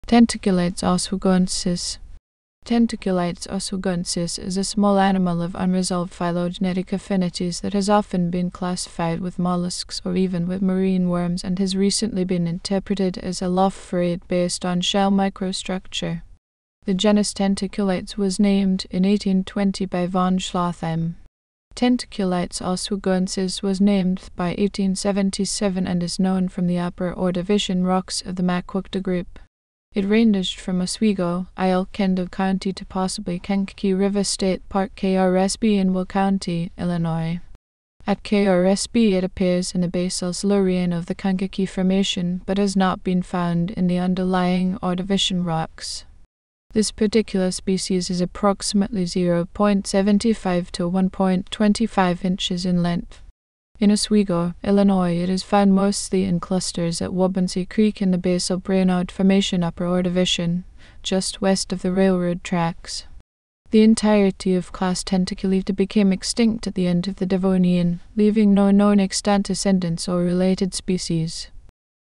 Tentaculites oswegoensis
Tentaculites oswegoensis is a small animal of unresolved phylogenetic affinities that has often been classified with molluscs or even with marine worms and has recently been interpreted as a lophophorate based on shell microstructure. The genus Tentaculites was named in 1820 by von Schlotheim. Tentaculites oswegoensis was named by 1877 and is known from the Upper Ordovician rocks of the Maquoketa Group. It ranged from Oswego, IL (Kendall County) to possibly Kankakee River State Park (KRSP) in Will County, Illinois. At KRSP it appears in the basal Silurian of the Kankakee Formation but has not been found in the underlying Ordovician rocks.
This particular species is approximately 0.75 to 1.25 inches in length. In Oswego, Illinois it is found mostly in clusters at Waubonsie Creek in the basal Brainard Formation (Upper Ordovician), just west of the railroad tracks. The entirety of Class Tentaculita became extinct at the end of the Devonian, leaving no known extant descendants or related species.